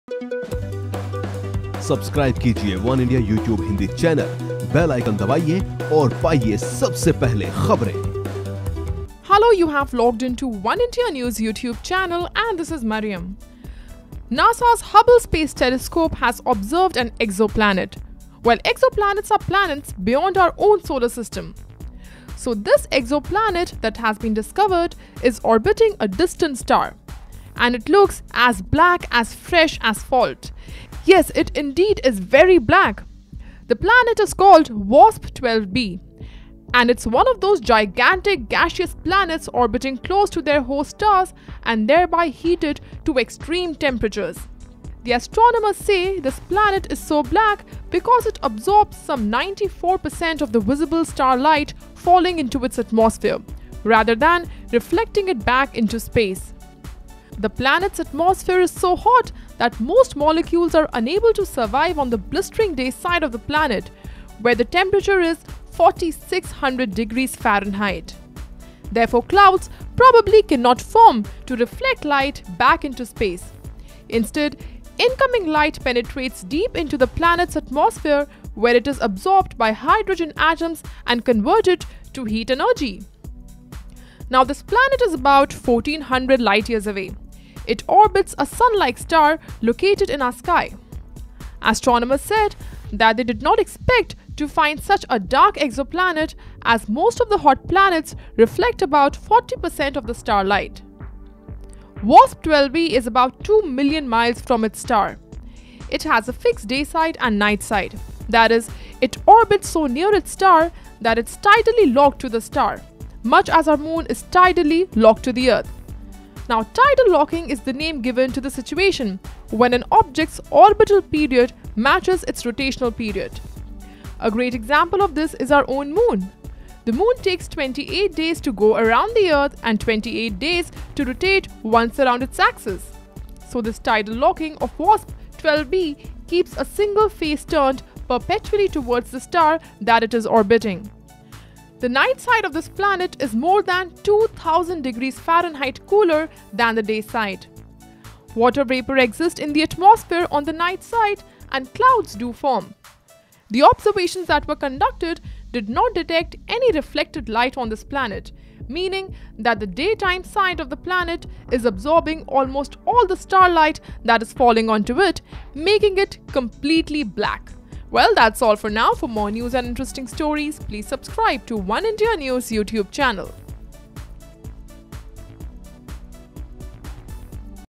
Astronomers discover a pitch black planet 1400 light years away | Oneindia News
NASA's Hubble Space Telescope has observed an exoplanet orbiting a distant star – that looks as black as fresh asphalt. The planet is called WASP-12b, and it’s one of those gigantic, gaseous planets orbiting close to their host stars and thereby heated to extreme temperatures.